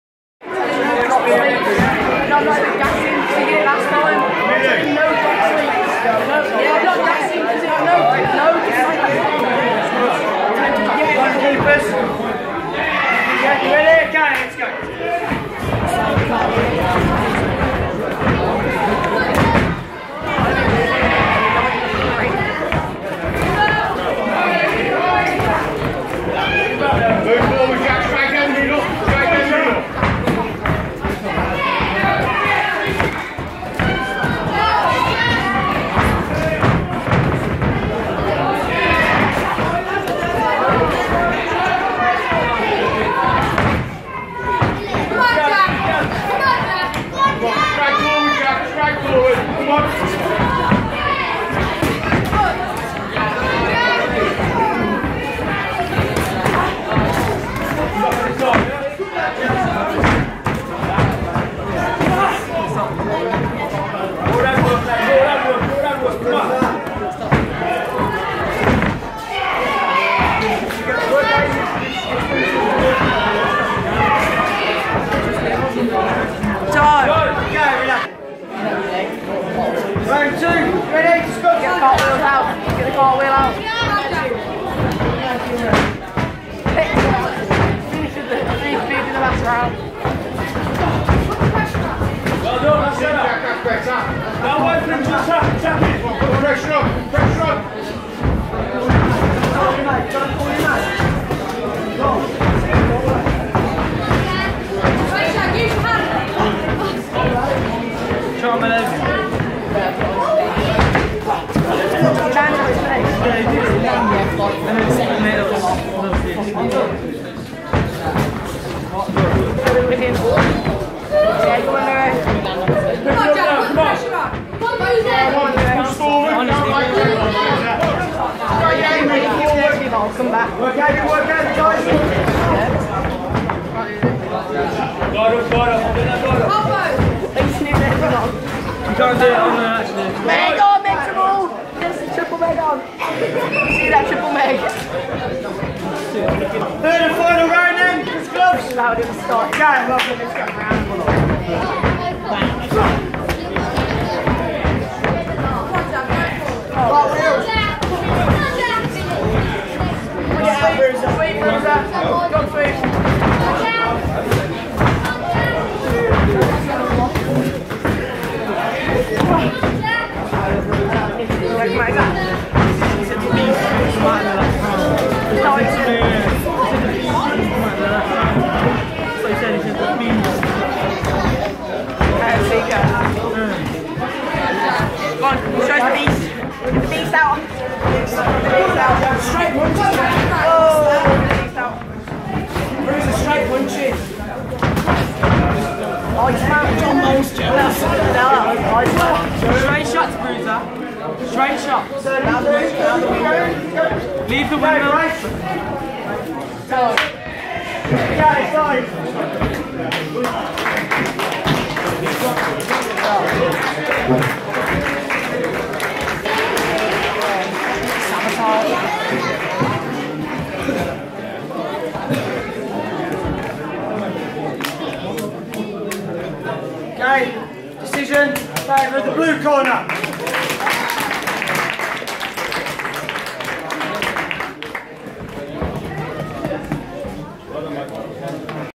Bruiser fight @ interclub
Bruiser (age 14) v 20 yr old